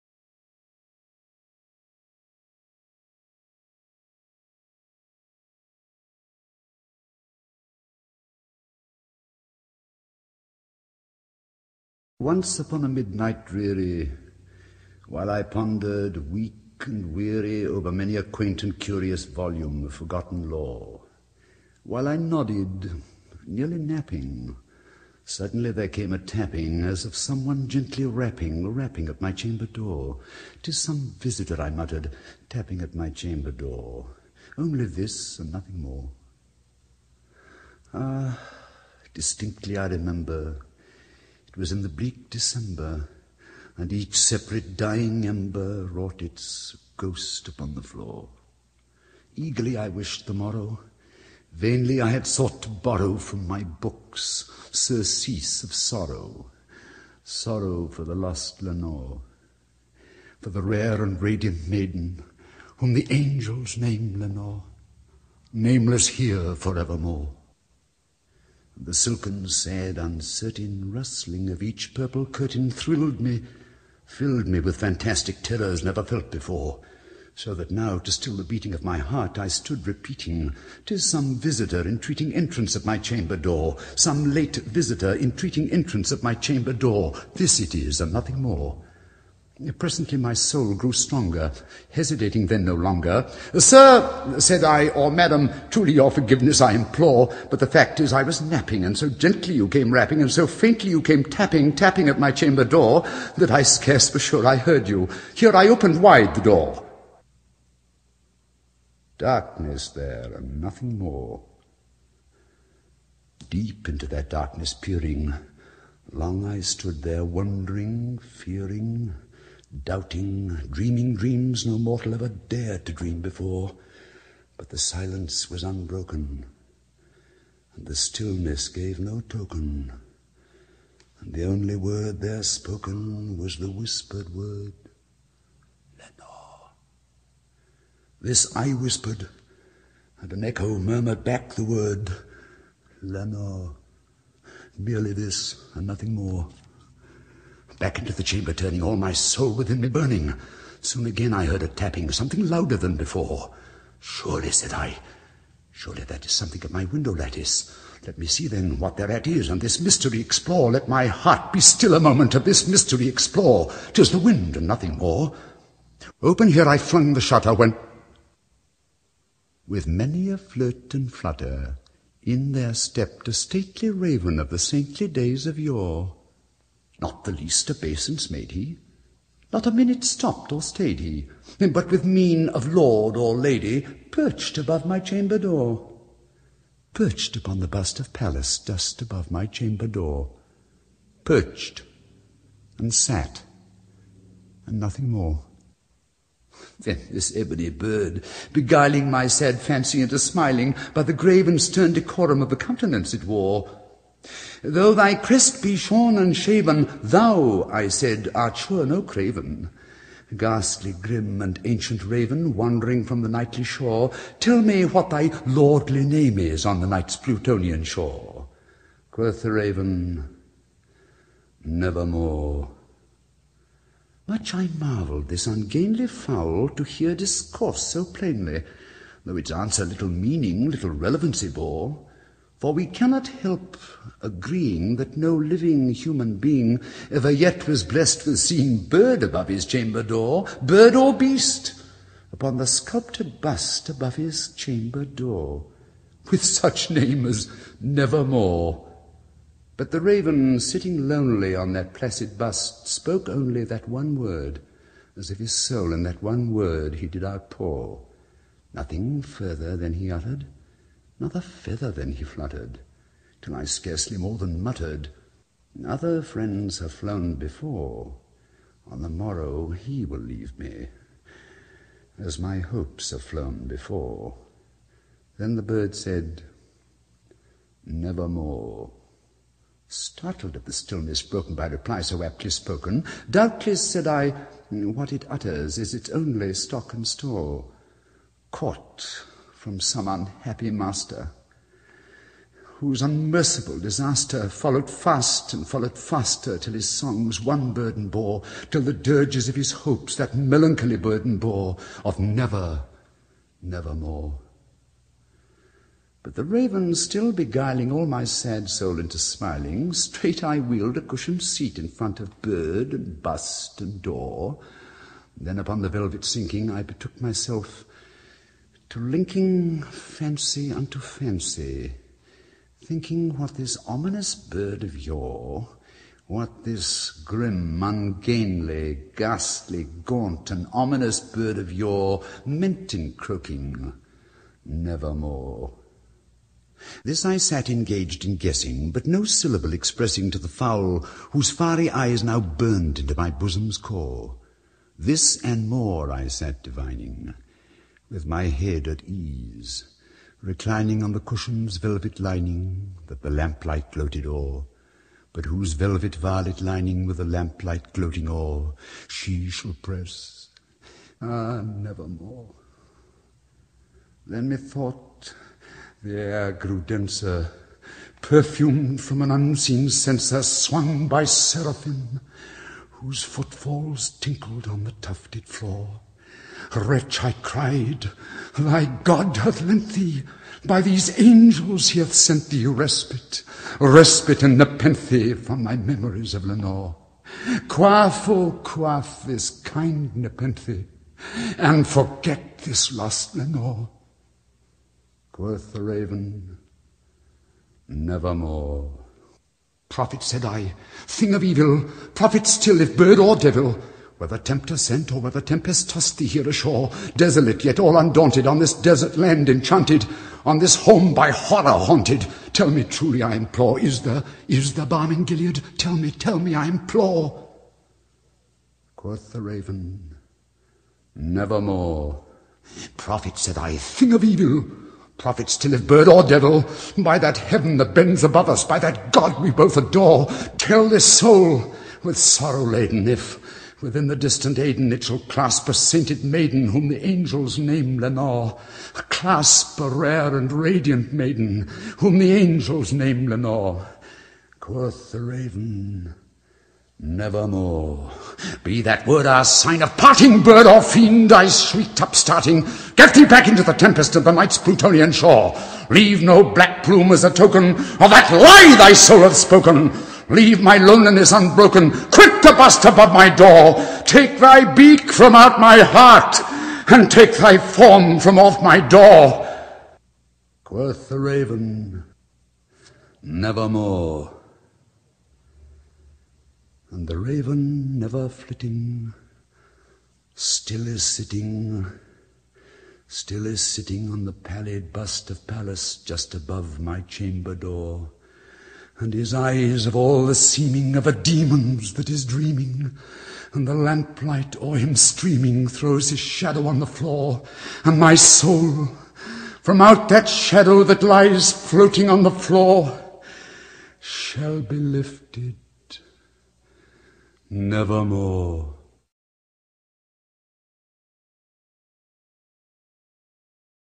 The Raven
I just decided to play with some Dore illustrations I had sitting on my hard drive and the great Basil Rathbone's reading of the poem. I wish I'd been able to get better resolution images, but it's just a bit of diddling while on break. If you find it useful or fun, wonderful.